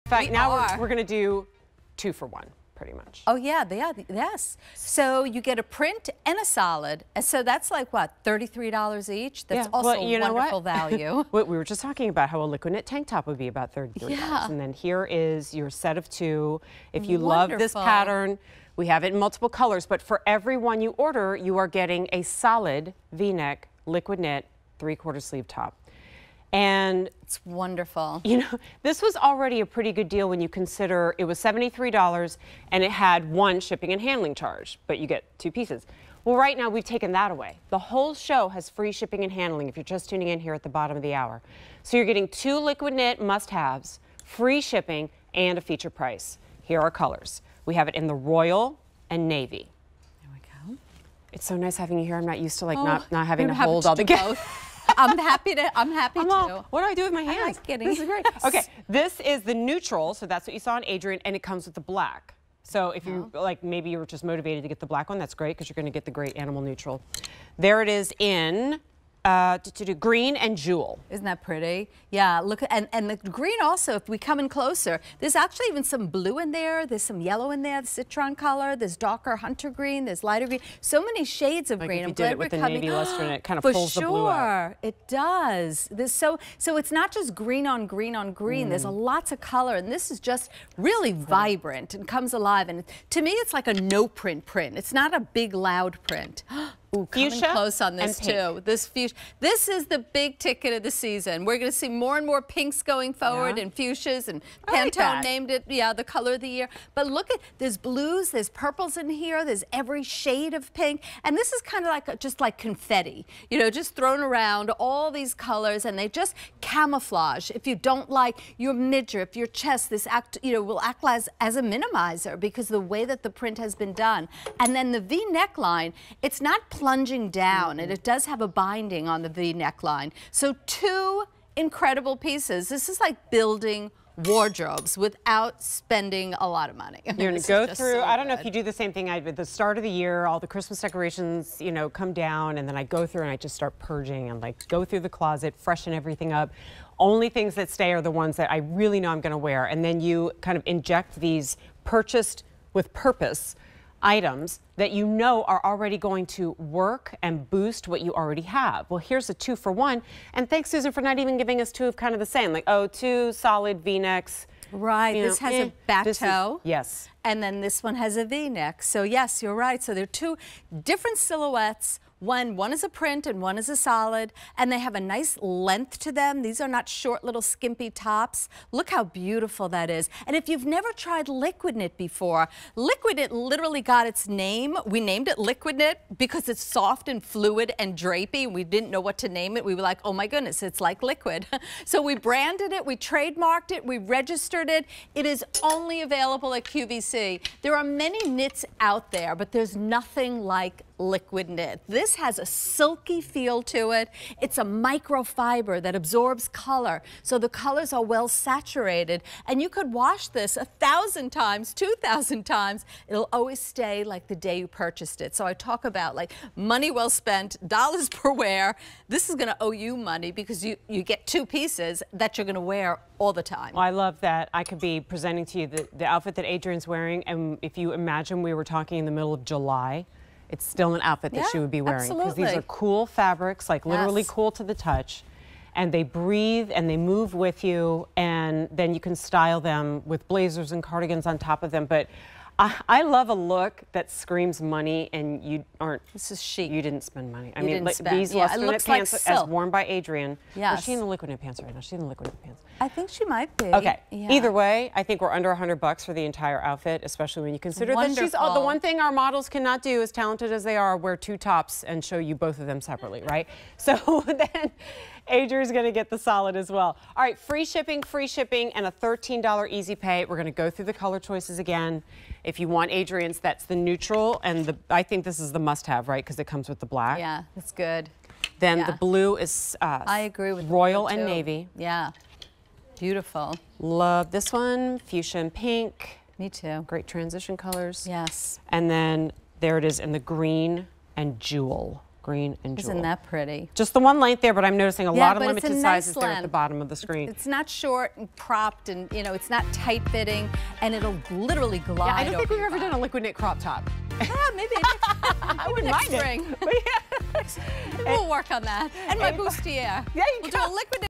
Susan Graver Printed & Solid Liquid Knit A-Line Top Set on QVC
Susan Graver Printed & Solid Liquid Knit A-Line Top Set

What's better than one Liquid Knit&reg; top? How about two? This set of soft, figure-flattering shirts include a print and a solid -- perfect for wearing with your favorite pair of jeans and a cute boot. From Susan Graver.